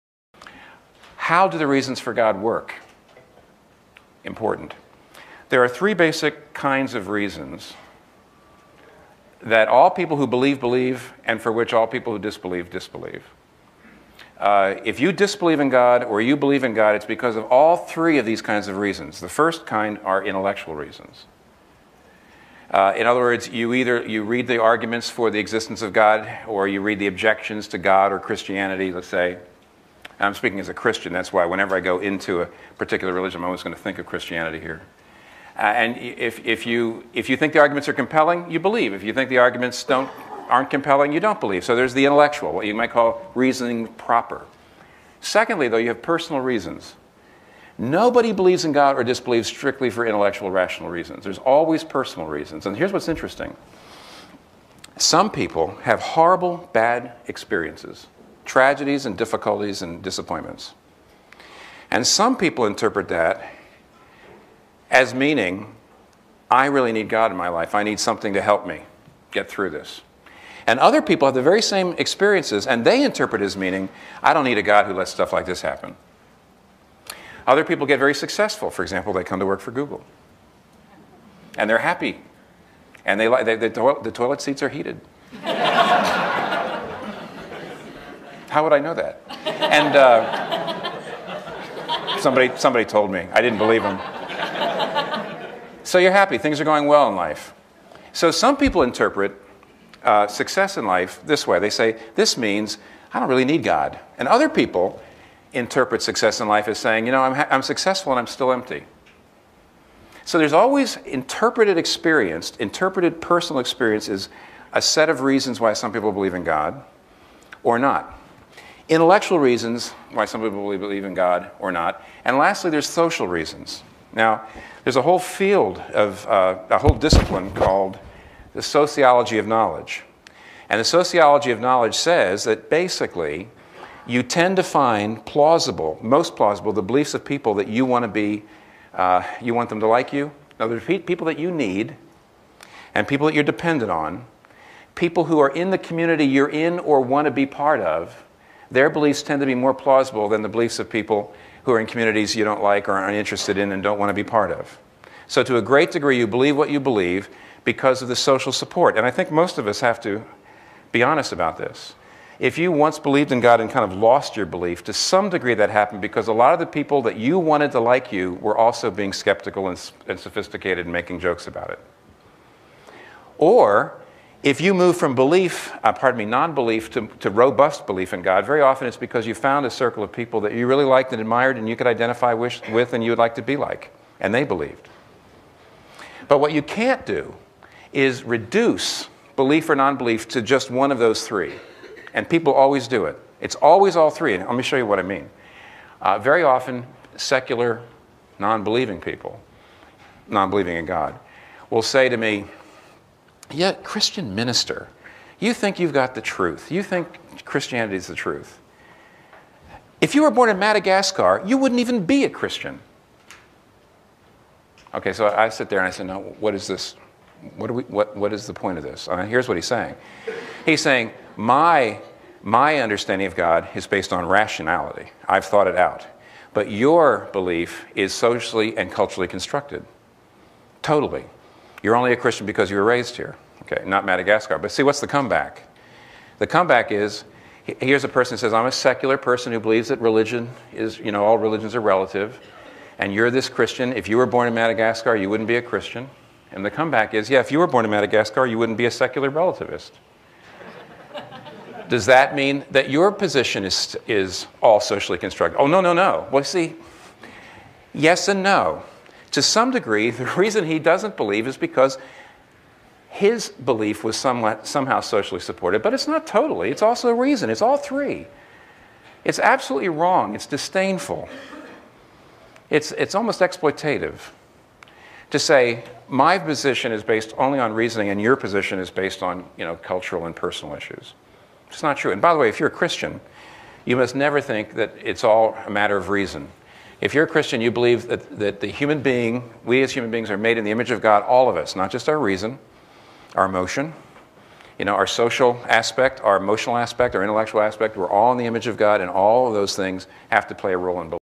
Reasons for Belief/Disbelief in God - Dr. Timothy Keller - Clip
This video is an excerpt from Dr. Timothy Keller's lecture Talks at Google titled "The Reason for God" (which is a summary lecture based off of his book with the same title.) I recommend watching the entire lecture in whole, but I find this answer interesting because it is a very common hypocritical objection. Here is the link to the full lecture